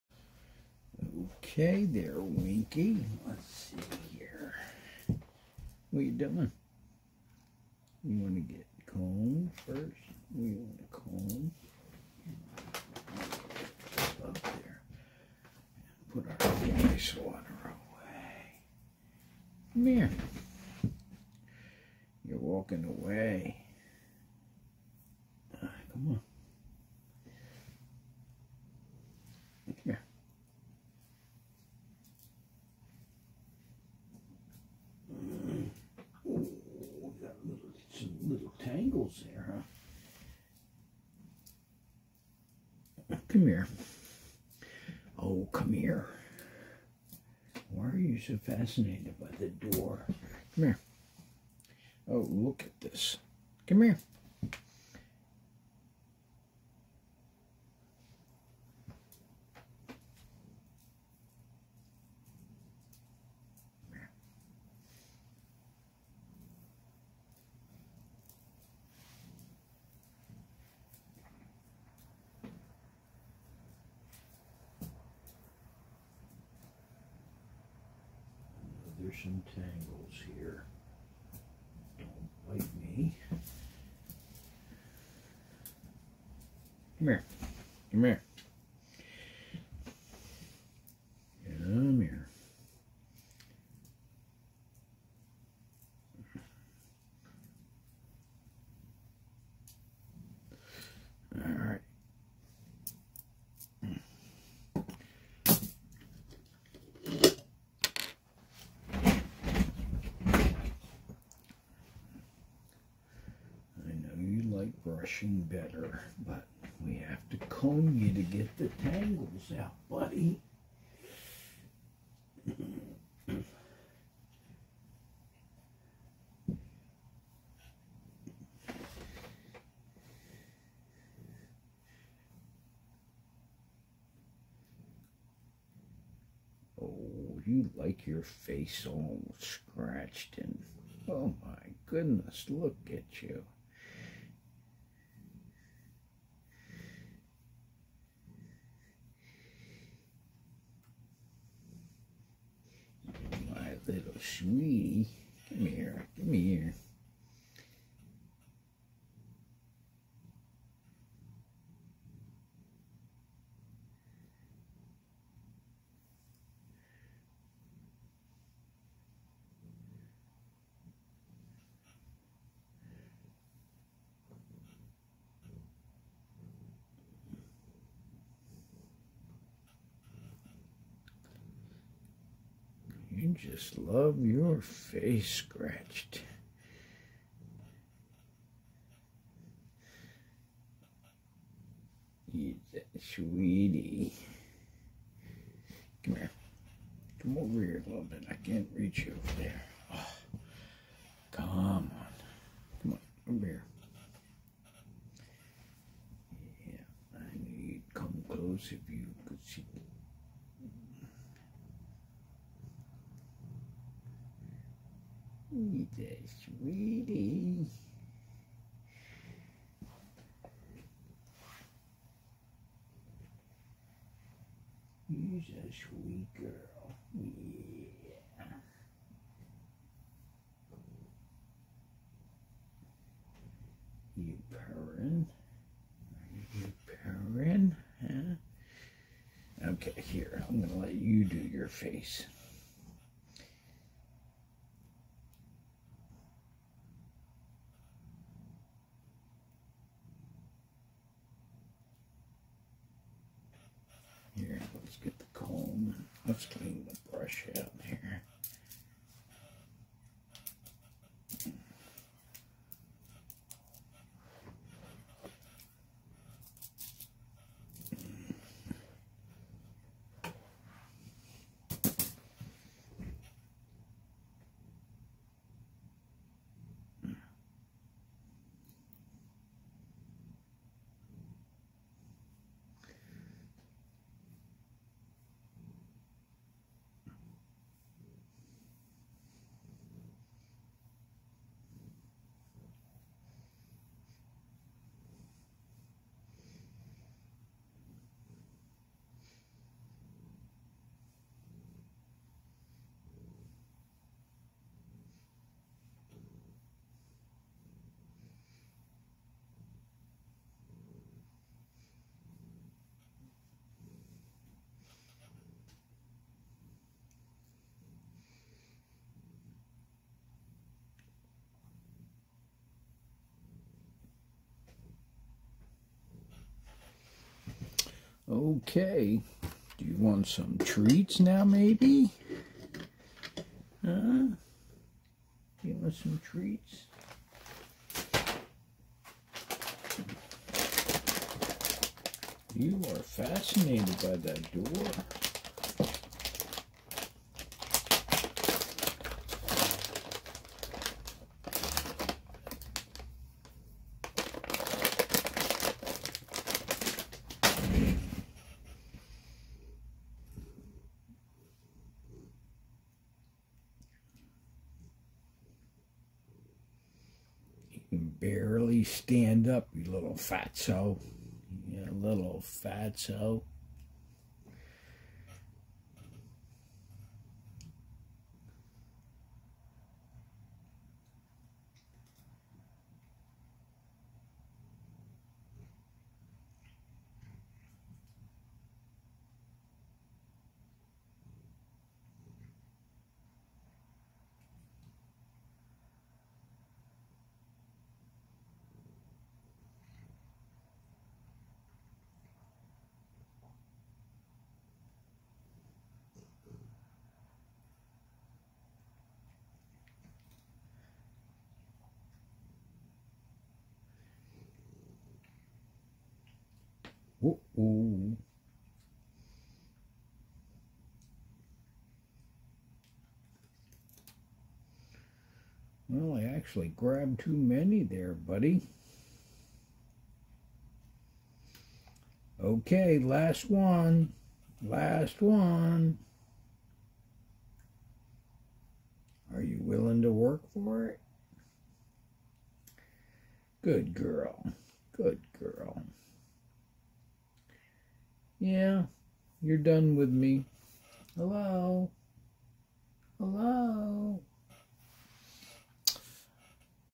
Winky's daily combing and brushing and of course treats
Haven't post any video of Winky in probably a year.  Her contract states I must give her more air time,  here it is. LOL  She gets her brushing every day.  When I have had a neighbor watch winky when away, he brushes her also, by choice as she loves it.